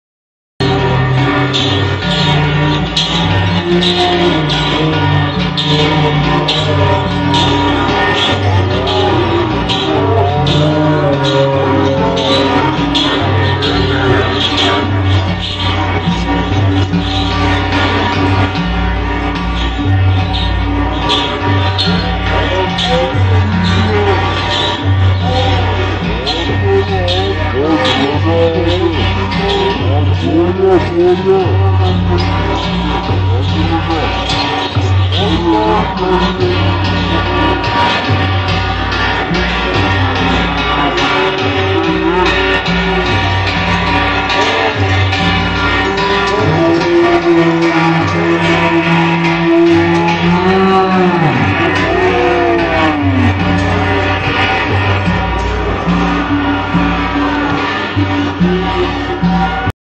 ऐसा होता है गवरी का शेर राजस्थानी गवरी में अम्बा माता के नार की जलक ...
गवरी का शेर. भील की यारी शेर की सवारी से कम नहीं है ..शेर से हाथ मिलाना .हर किसी का काम नहीं .गवरी सेमा gavari sema
.शेर को छुकर  माँ गौरी का आशीर्वाद लेते ..